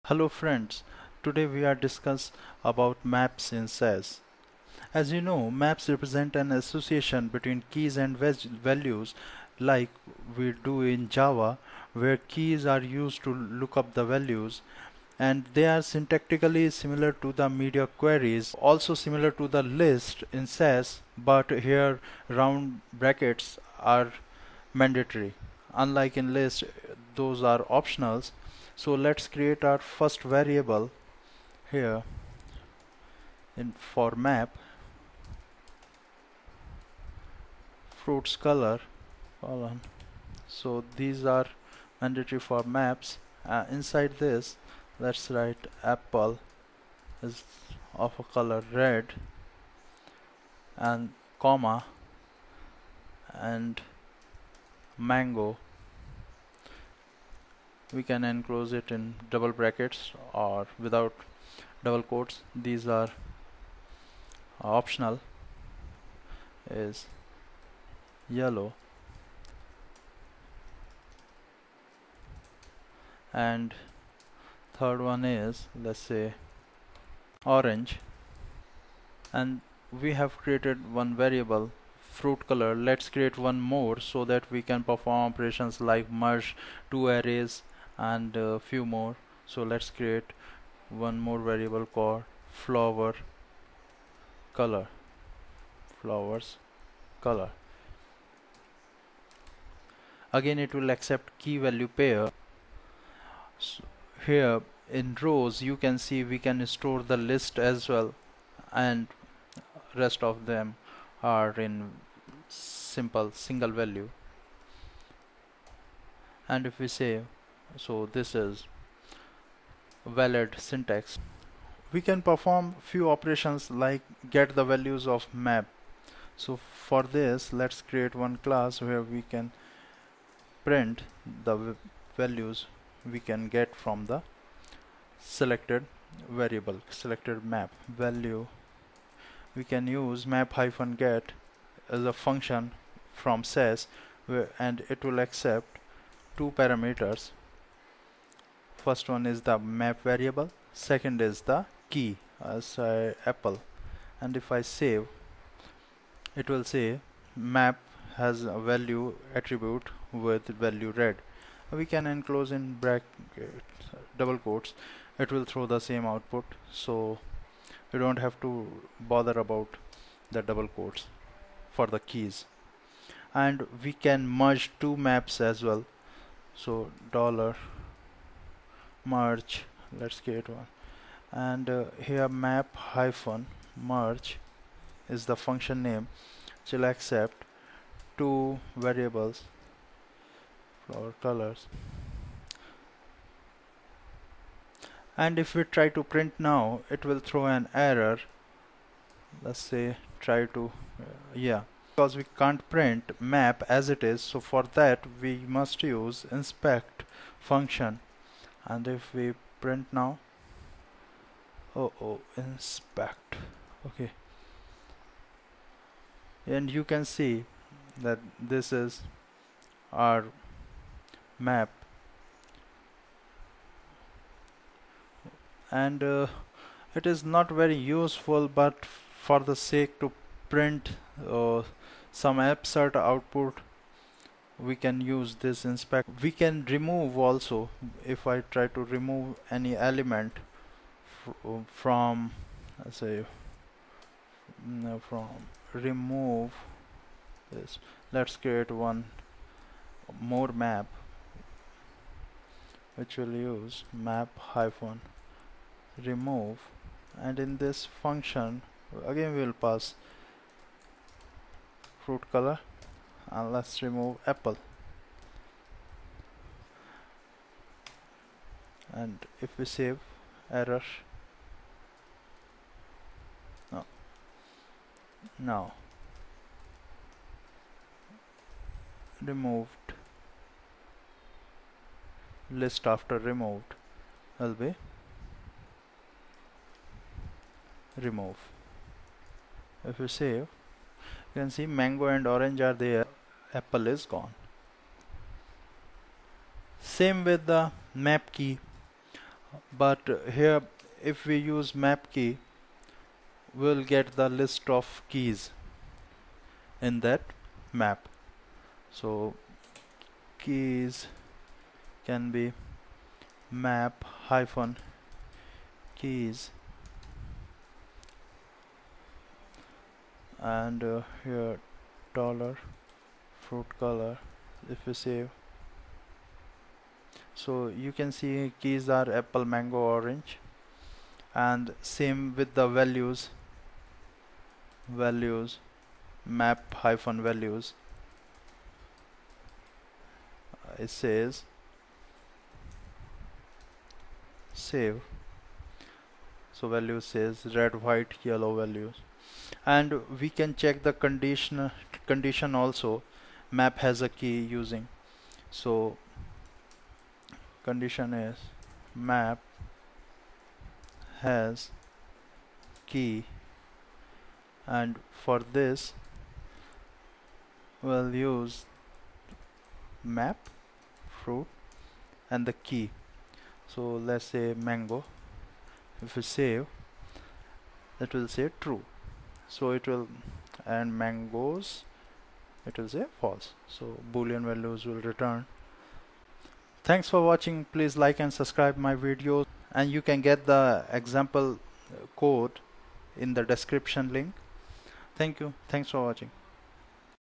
Maps in SASS: SASS Tutorials #6
Maps functions in SASS.

map-get($map, $key)
map-merge($map1, $map2)
map-remove($map, $keys…)
map-keys($map)
map-values($map)
map-has-key($map, $key)
keywords($args)